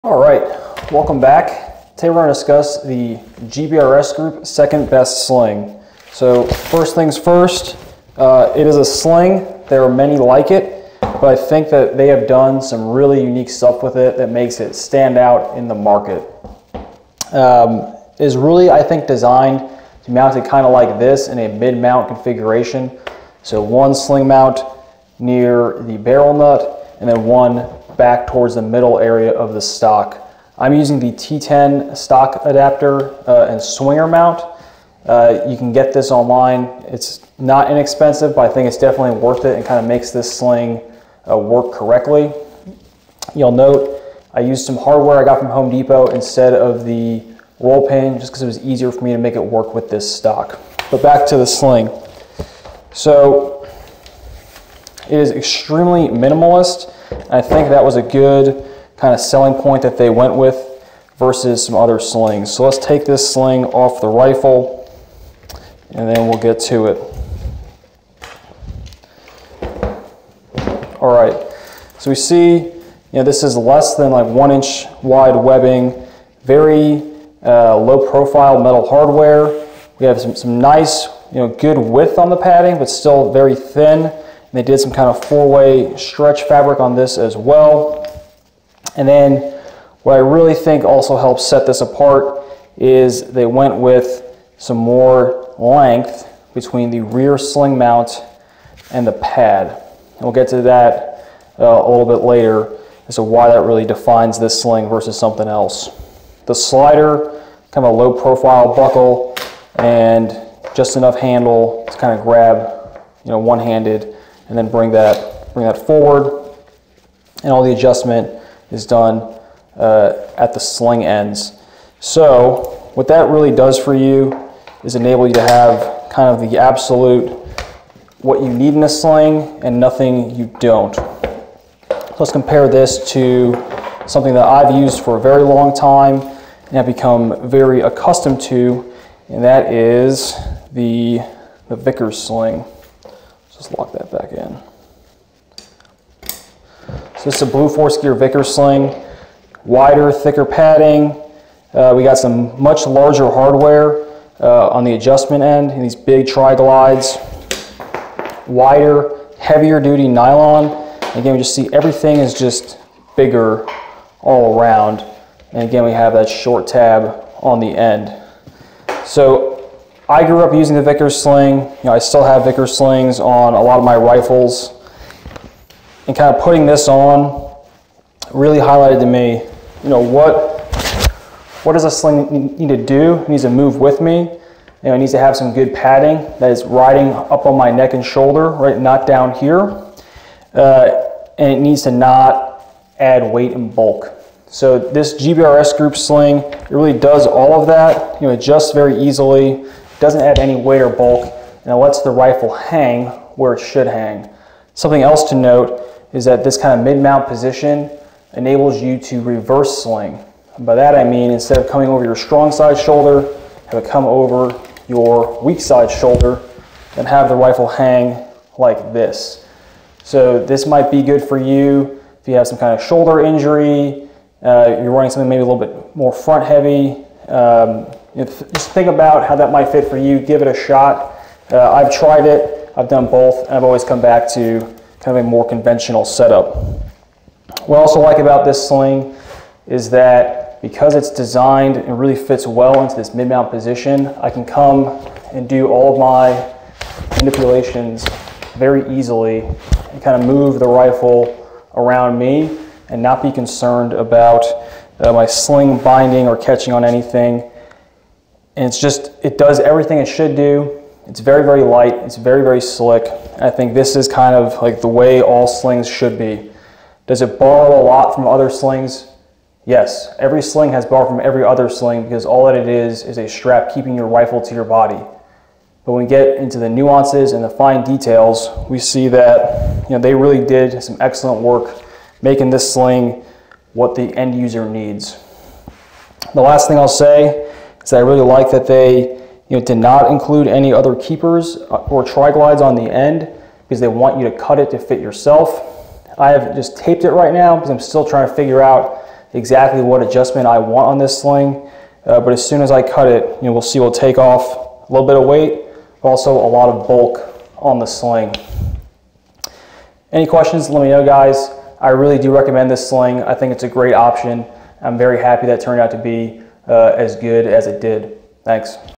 GBRS Group SBS: Second Best Sling Overview
Quick overview of a great new sling from GBRS Group.